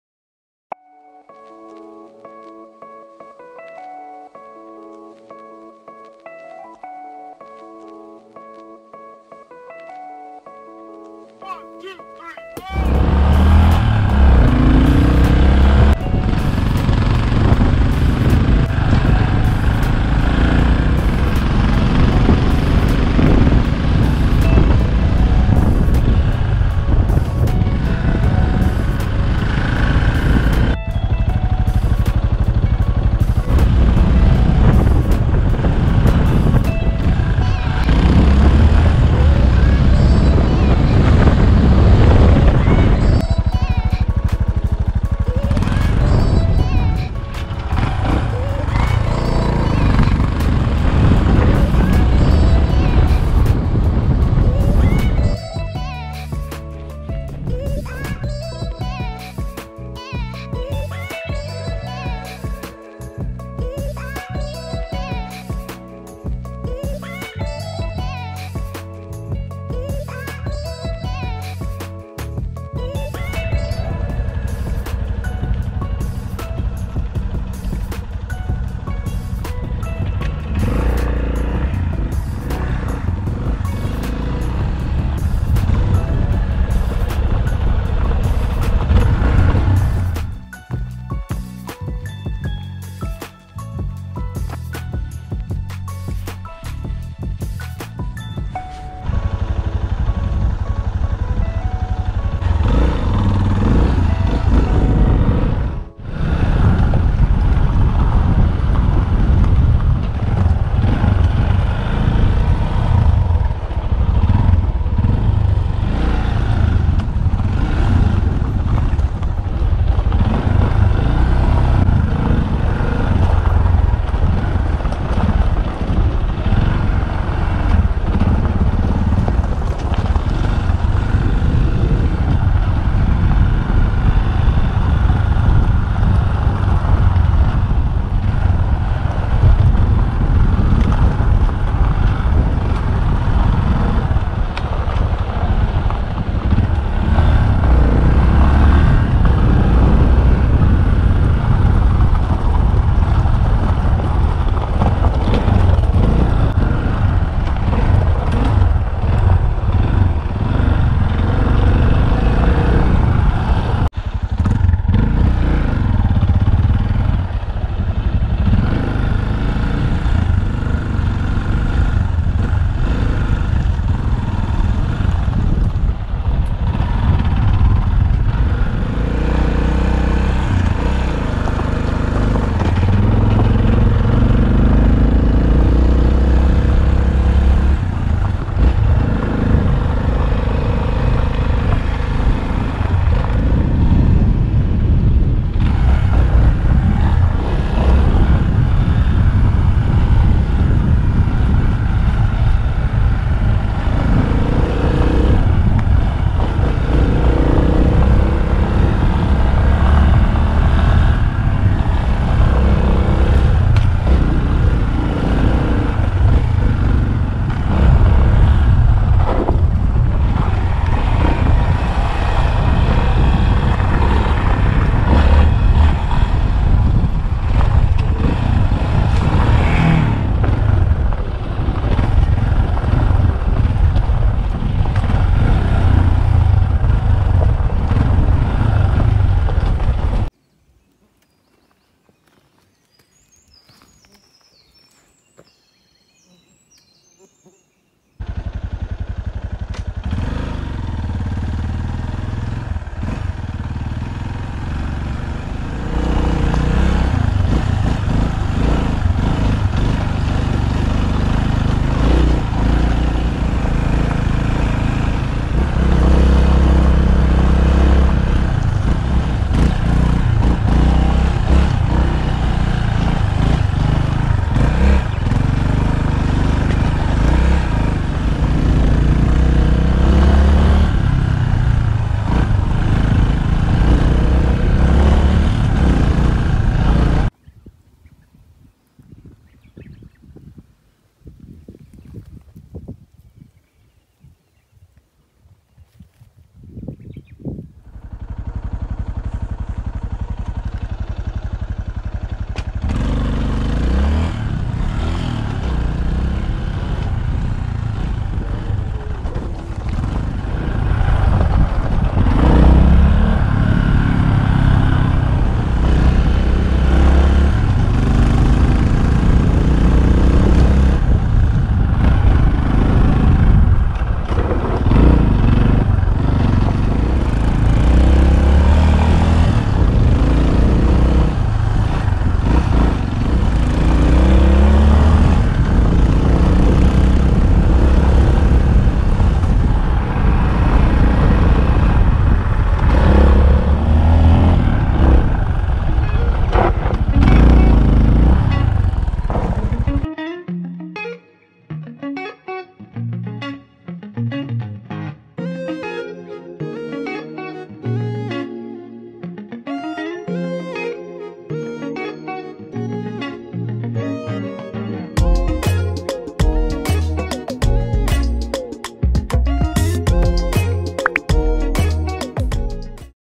天空の林道【中津明神山】トリッカーでアタック(ロングバージョン)
高知県と愛媛県の県境にある中津明神山。そこから伸びる天空の林道。走行車両はヤマハ・トリッカーです。
愛媛県西条市
ライダースクラブはなやま